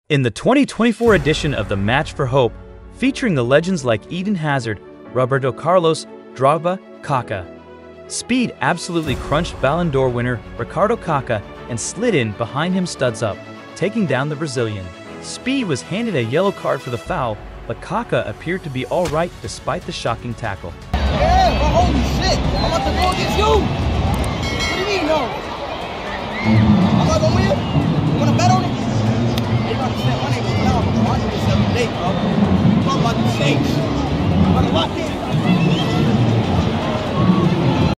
ISHOWSPEED SLIDE TACKLED KAKA & GETS A YELLOW CARD (Almost Injured Kaka)
ISHOWSPEED SLIDE TACKLED KAKA & GETS A YELLOW CARD IN MATCH FOR HOPE